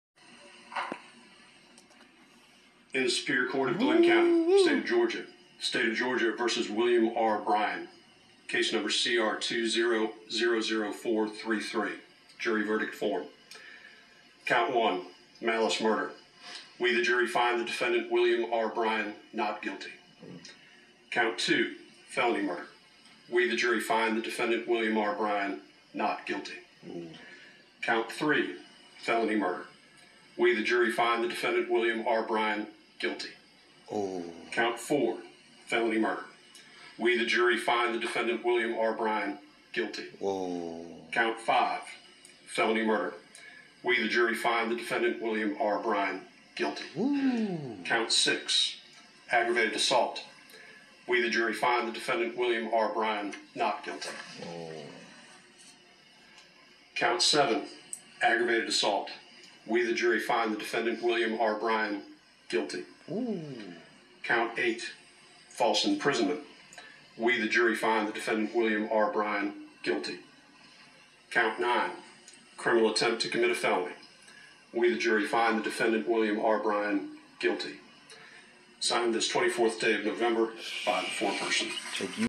WilliamRoddieBryan | "Guilty" of AuhmaudArbery Murder...
A video of the incident was recorded by William "Roddie" Bryan, a neighbor of the McMichaels, using his cellphone from his vehicle as he followed Arbery jogging down the neighborhood road.  The video showed Arbery jogging on the left side of the road when he encountered a white pickup truck that had stopped in the right lane.  Gregory McMichael is standing in the truck bed, while Travis McMichael initially stands beside the driver's door with a shotgun.  Bryan's vehicle comes to a stop behind Arbery and the pickup truck.